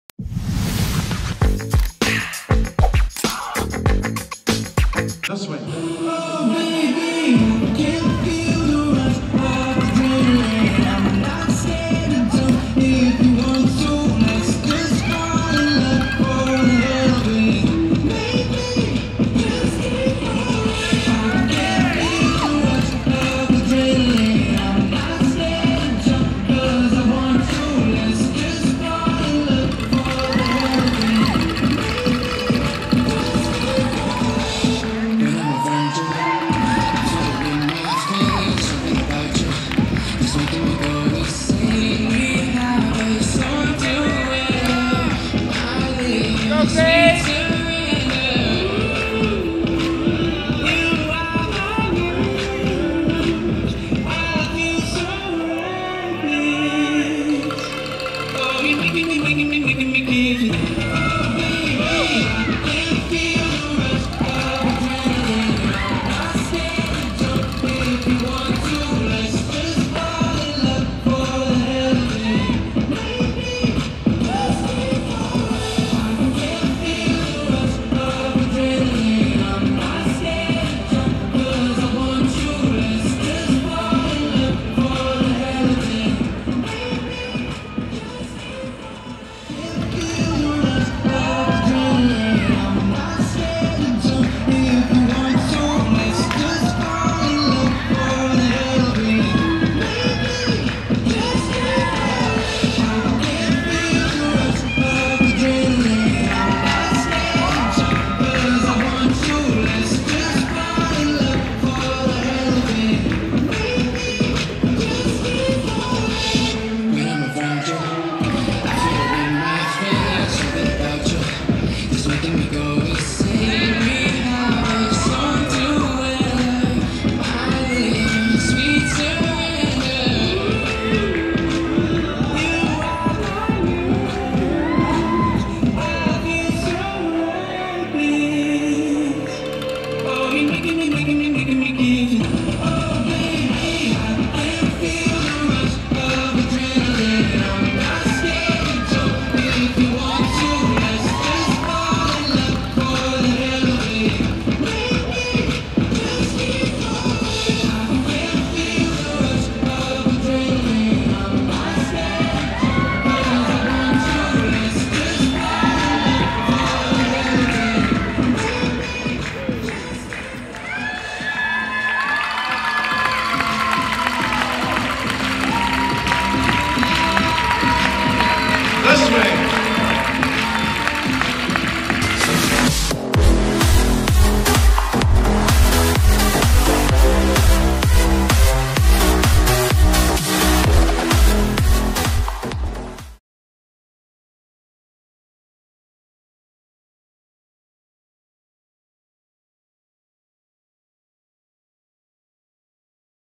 Swing I Professional American Rhythm I Fred Astaire National Dance Championships 2020
Open Professional American Rhythm Championship - QF - Swing

Thank you to the organizer Stephen Knight of Fred Astaire National Dance Championships (NDC) 2020 for inviting Panache Star Video to cover & promote this wonderful event!